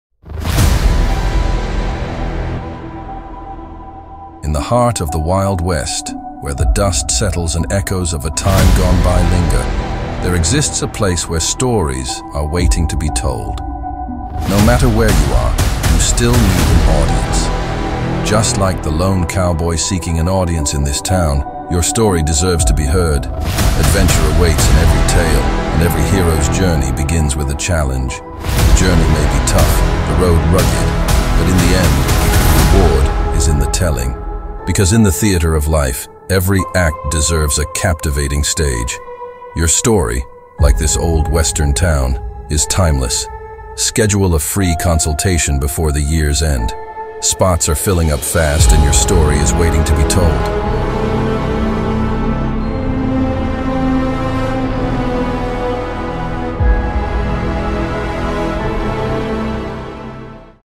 𝗗𝗘𝗠 𝗩𝗶𝗱𝗲𝗼 𝗣𝗿𝗼𝗱𝘂𝗰𝘁𝗶𝗼𝗻𝘀 - Ditching Stock for Soul: Elevating Video Quality with Live Action and Sound!
Audio plays a significant role in shaping how content is perceived; background music and narration can elevate or diminish the overall impact of a video.

AI-generated audio often falls short in conveying the subtleties of human emotion, leaving viewers feeling disengaged.

Professional voiceovers infused with genuine passion breathe life into stories, while thoughtfully selected soundtracks enhance entertainment value by matching tone and pace to visuals seamlessly.

By prioritizing these elements over mere efficiency, we create more compelling narratives that capture attention and evoke genuine reactions from our audience.

Interested in using AI in your Video Marketing Campaigns:
* Storytelling - content creation
* AI Voice-over
* Live 4K Video
* Audio soundtrack